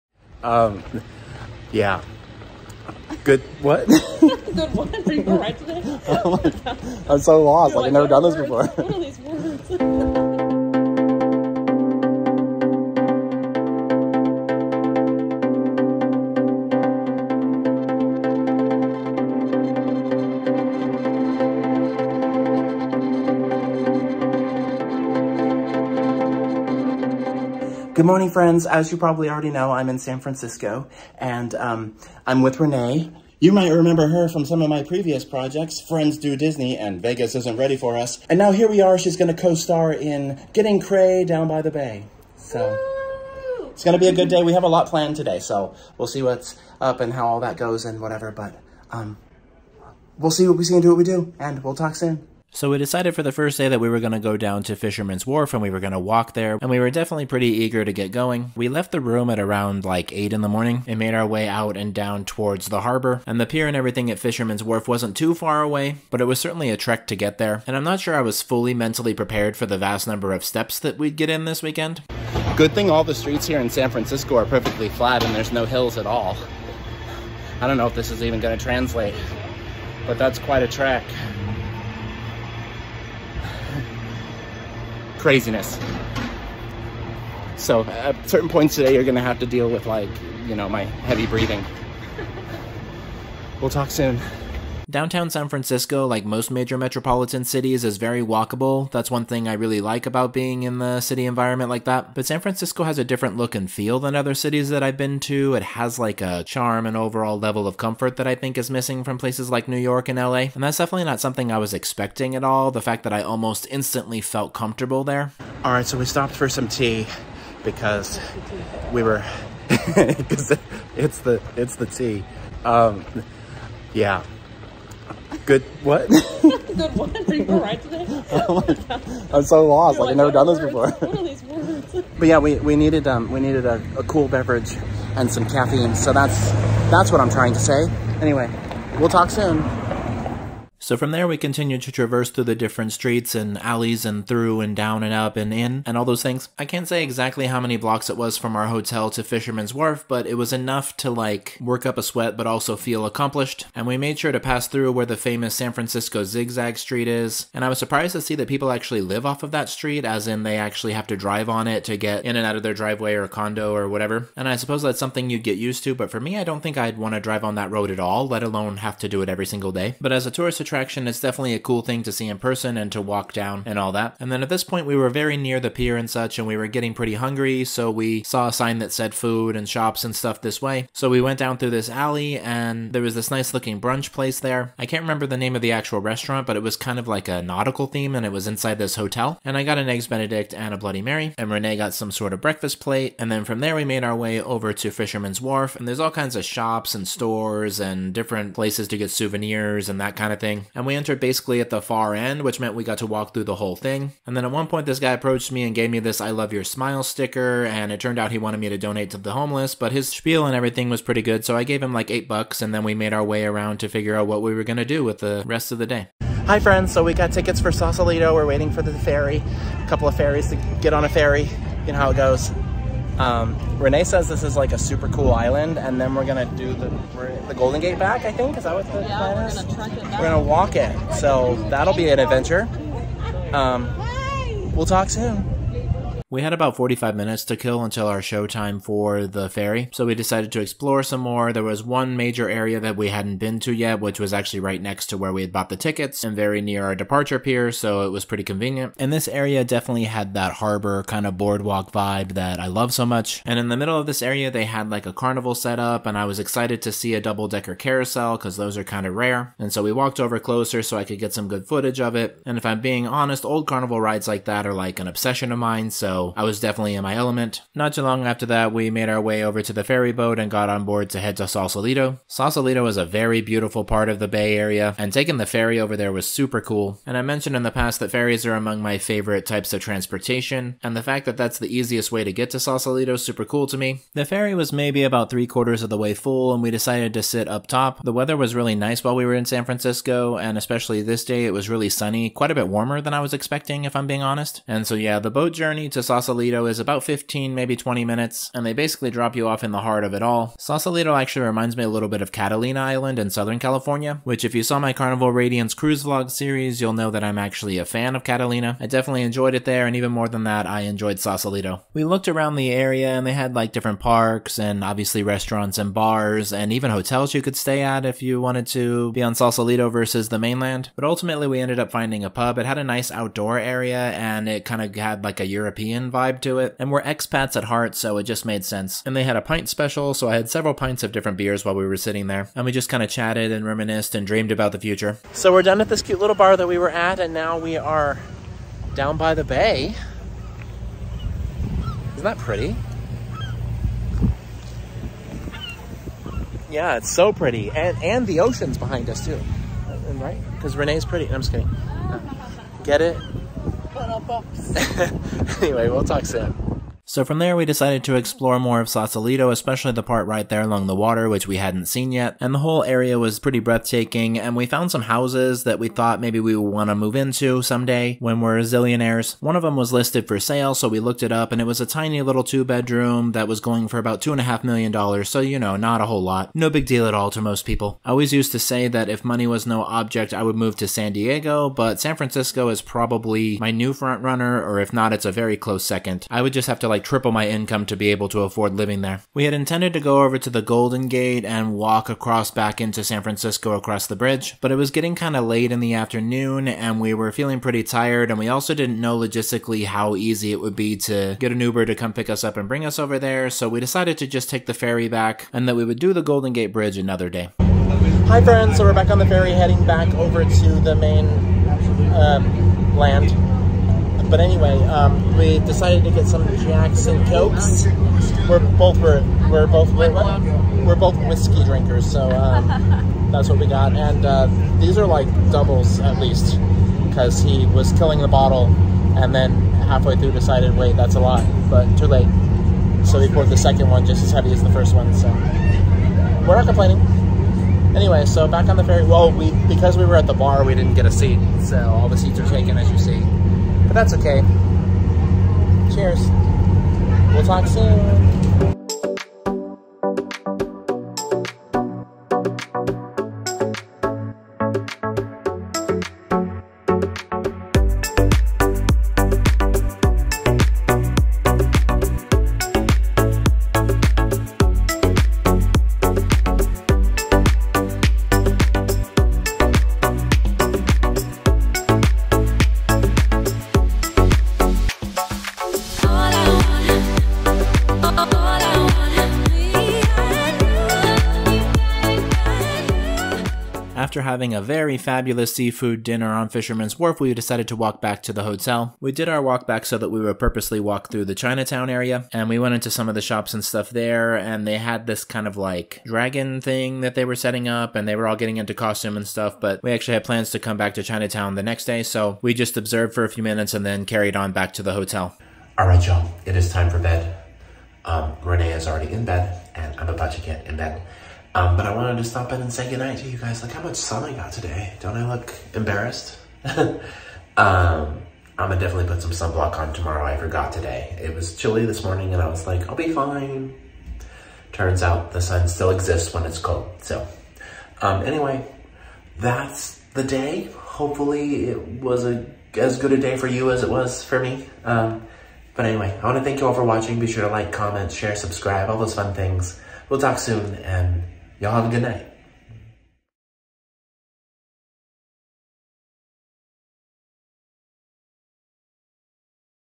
A walk to Fisherman's Wharf, and a ferry to Sausalito... || San Francisco on a Whim, August 2022
About 3 weeks before her SanFrancisco concert, my friend Renee told me that she would be in the BayArea, and that I should totally join her for a weekend getaway. I checked flights, and by some miracle, they weren't an arm and a leg. So...I booked it and the rest is history. In this second episode, we leave the hotel early, and head to Fisherman's Wharf. We then catch the ferry over to Sausalito, before having dinner at Pier 39. Comments are welcome!

SAN FRANCISCO TRIP: 8/26/2022 to 8/29/2022

Twitter—@tbbevis
Instagram—@tbbevis